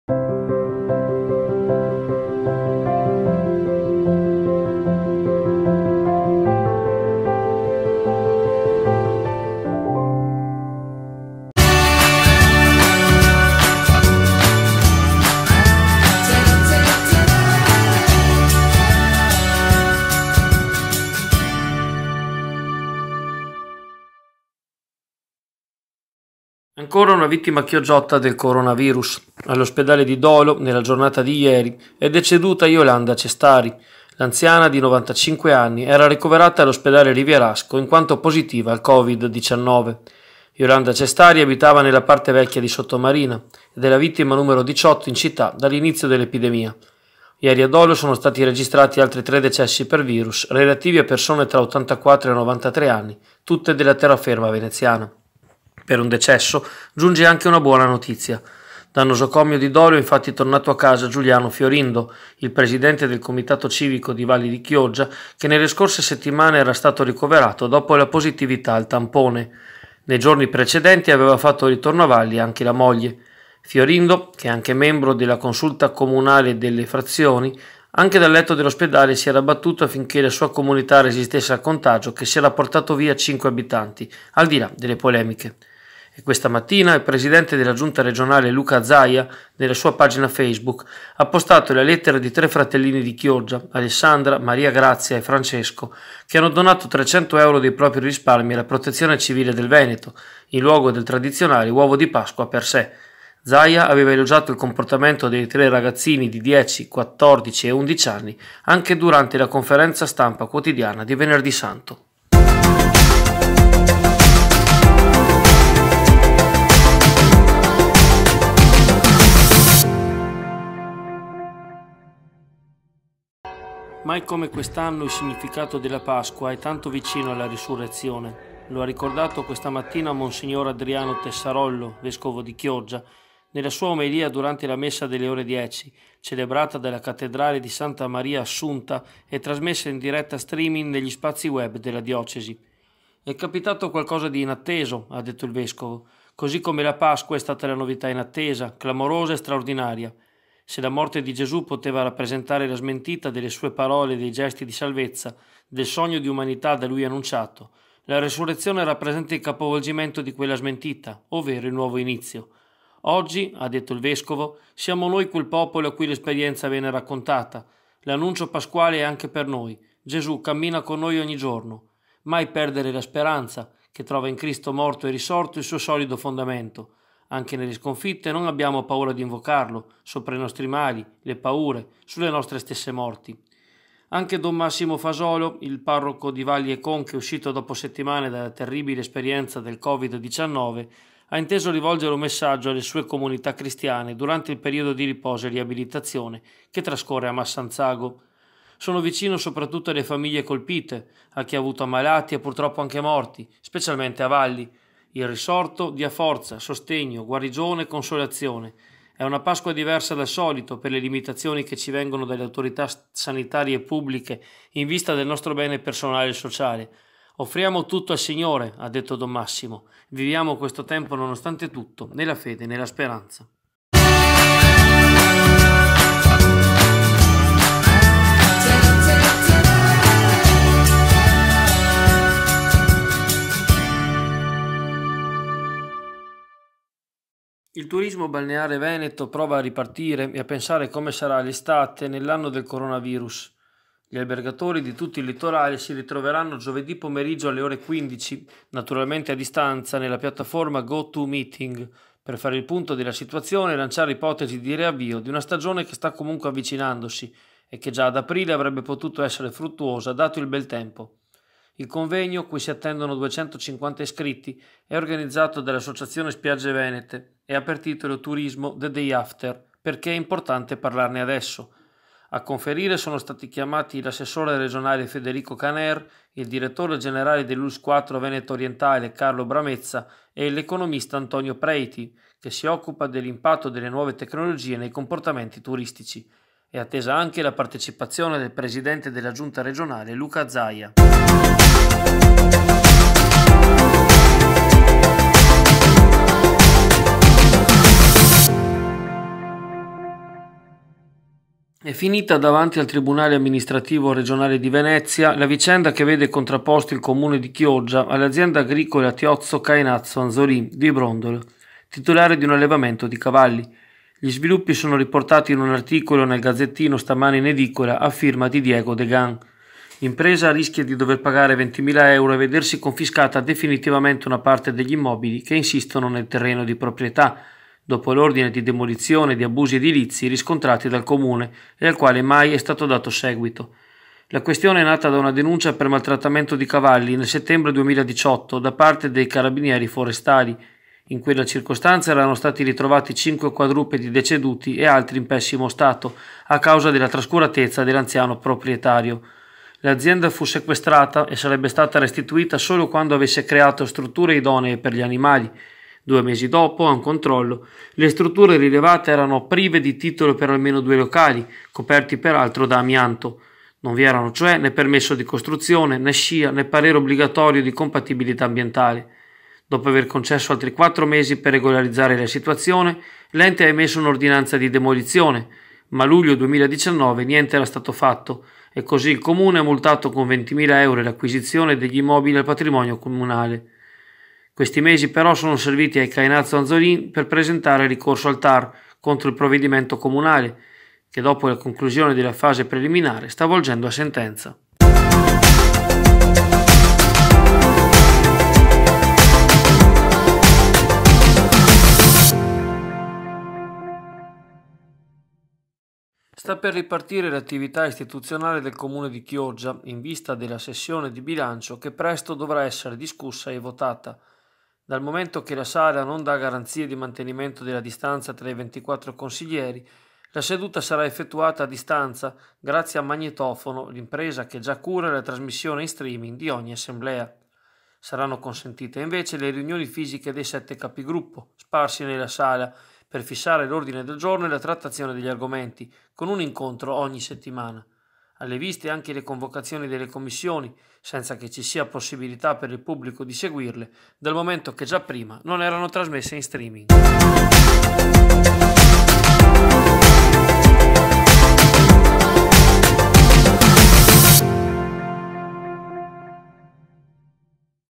TG AZZURRA: RESURREZIONE, IL VESCOVO E DON MASSIMO APRONO LE PORTE ALLA SPERANZA - 104/20, 12 aprile
Nel numero odierno:

- ieri all'ospedale di Dolo la diciottesima vittima chioggiotta del virus. Zaia commosso dalla donazione di tre ragazzini → l'articolo continua su

- Pasqua di Resurrezione, il vescovo Tessarollo nella messa e don Massimo uscito dal virus aprono le porte alla speranza → l'articolo continua su

- giovedì pomeriggio convegno a distanza fra gli albergatori del litorale veneto per pianificare la ripartenza turistica → l'articolo continua su

- il Comune ordina la demolizione di strutture abusive per allevamento di cavalli a Brondolo, l'azienda fa ricorso al TAR → l'articolo continua su

- consiglio comunale a distanza per l'approvazione del bilancio. Le commissioni capigruppo invece si terranno in municipio → l'articolo continua su